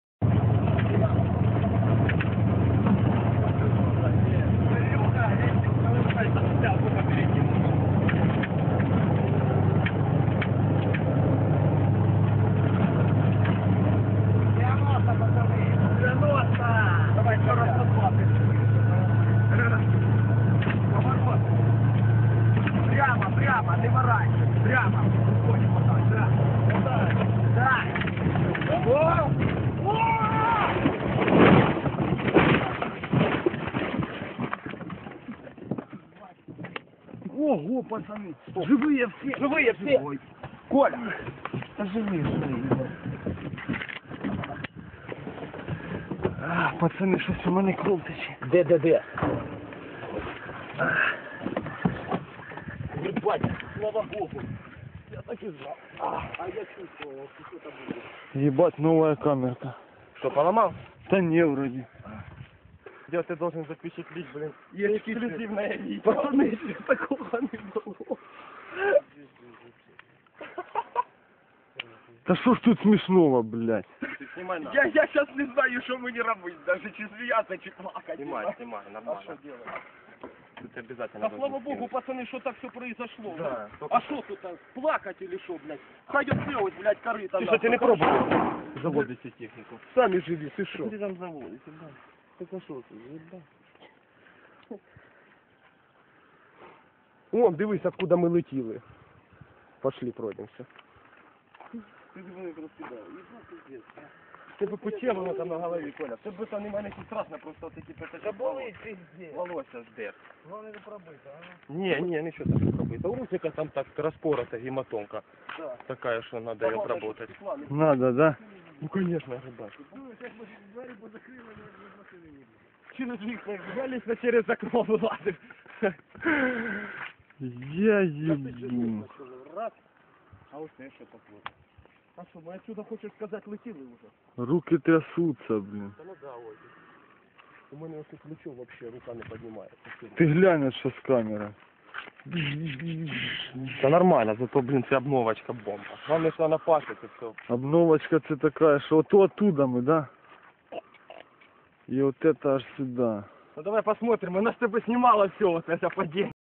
Девяноста, пацаны !
Guys from Ukraine are fuken lucky and prove that ZAZ is reliable and safe rally car ! :D

(not my vid, copyright to those guys)